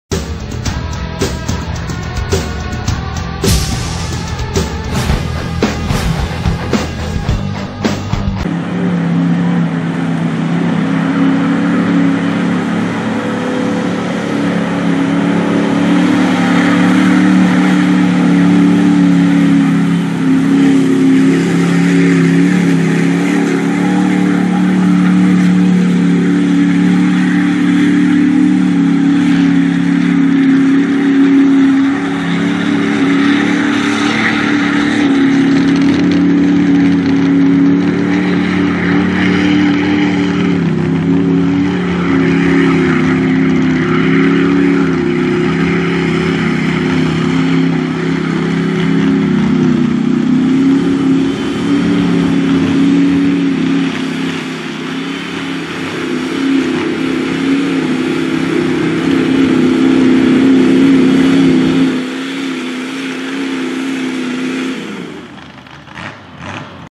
Chevy Pickup Mudding At Silver Bullet Mud Bog April 2015
Footage from Silver Bullet Speedway Mud Bog April 2015